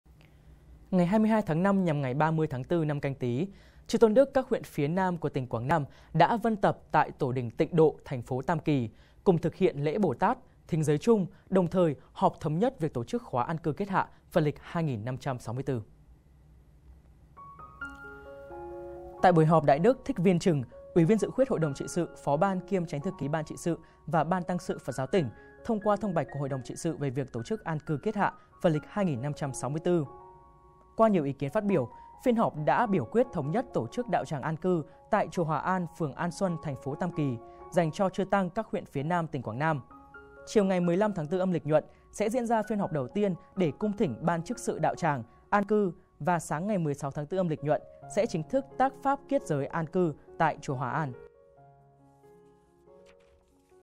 Quảng Nam: Chư Tăng thống nhất kiết giới An cư PL.2564.| An Viên 24h
Chư Tôn đức các huyện phía Nam của tỉnh Quảng Nam đã vân tập tại tổ đình Tịnh Độ, TP. Tam Kỳ cùng thực hiện lễ bố-tát, thính giới chung, đồng thời họp thống nhất việc tổ chức khóa An cư kiết hạ PL.2564.